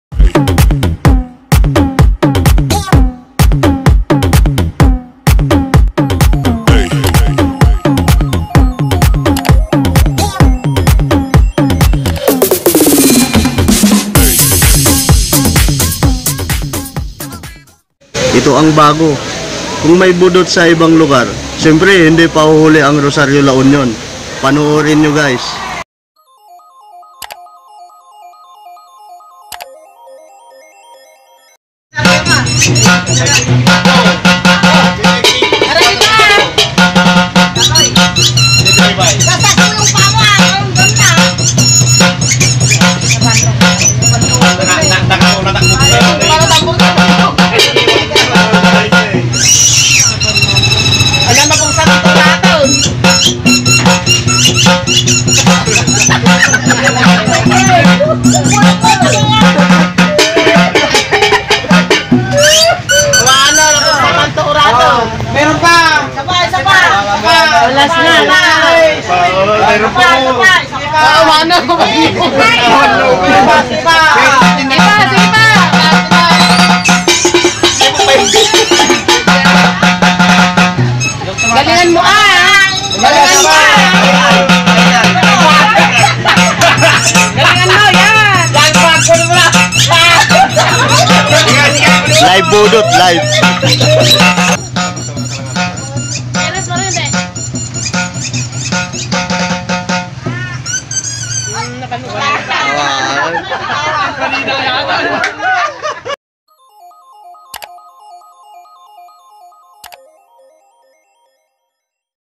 KUNG MAY BUDOT SA IBANG LUGAR HINDI PAHUHULI ANG ROSARIO LA UNION
Na daanan ko lng nag papatugtug pinagkaka guluhan kaya kinunan kuna ng video..